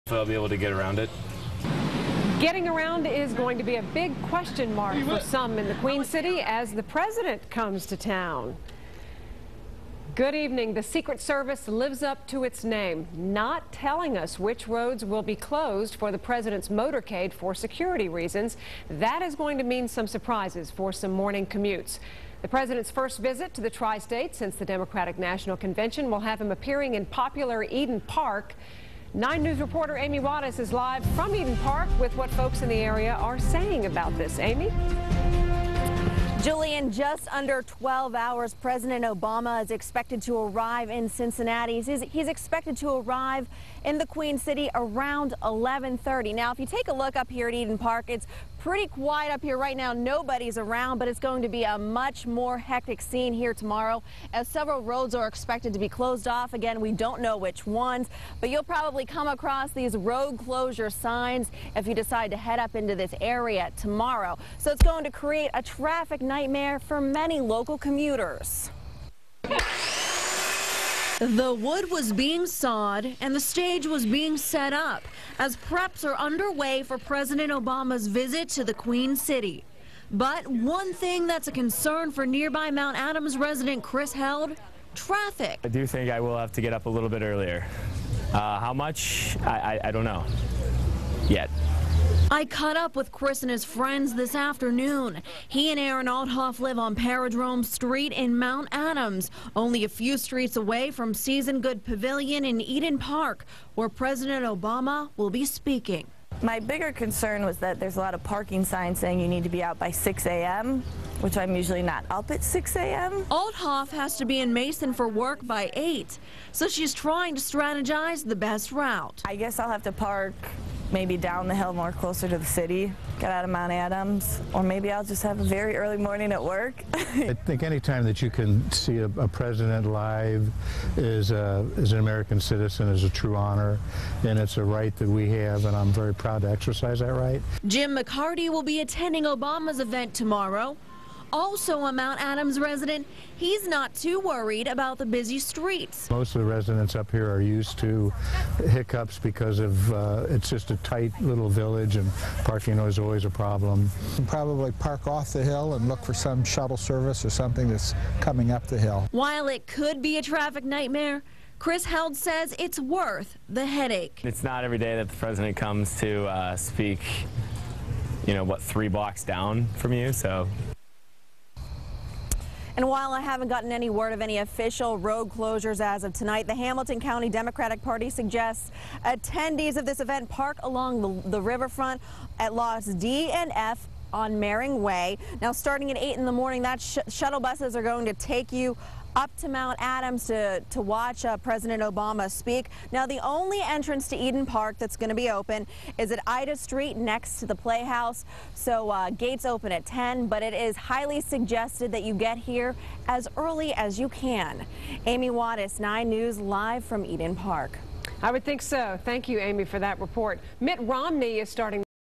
Traffic concerns for Obama visit
Residents in Mount Adams speak out about traffic concerns as President Obama makes a visit on Monday.